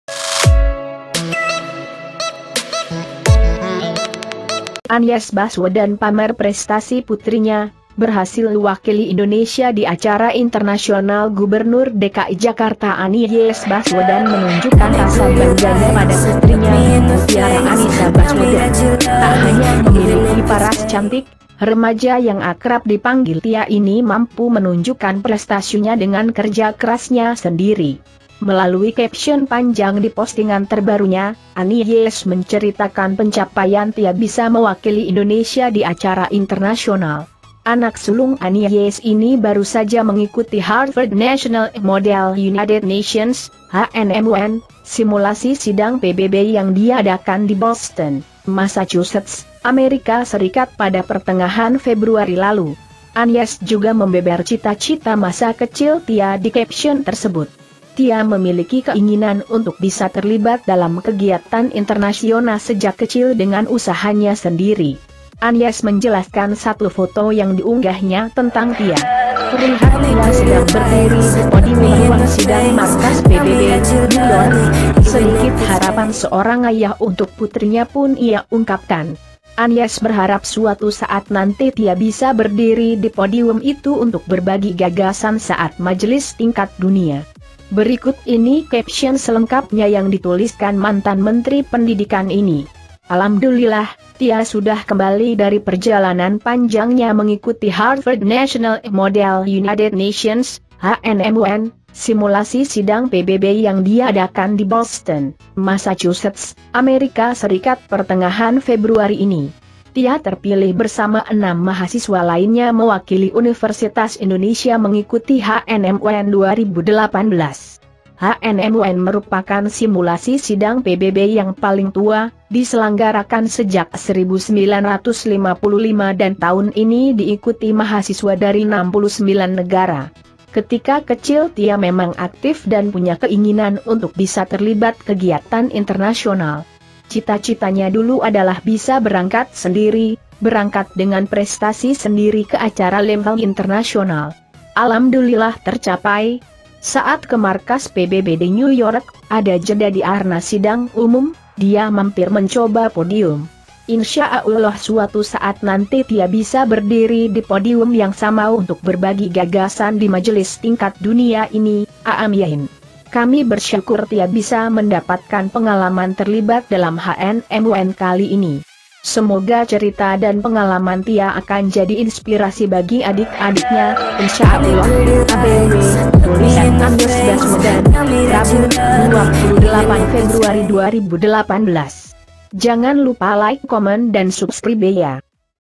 ANIES BASWEDAN PAMER PRESTASI PUTRINYA
Gubernur DKI Jakarta Anies Baswedan menunjukkan rasa bangganya pada putrinya, Mutiara Annisa Baswedan.

Tak hanya memiliki paras cantik, remaja yang akrab dipanggil Tia ini mampu menunjukkan prestasinya dengan kerja kerasnya sendiri.

Melalui caption panjang di postingan terbarunya, Anies menceritakan pencapaian Tia bisa mewakili Indonesia di acara Internasional.Anak sulung Anies ini baru saja mengikuti Harvard National Model United Nations (HNMUN), simulasi sidang PBB yang diadakan di Boston, Massachussets, Amerika Serikat pada pertengahan Februari lalu.

Anies juga membeberkan cita-cita masa kecil Tia di caption tersebut.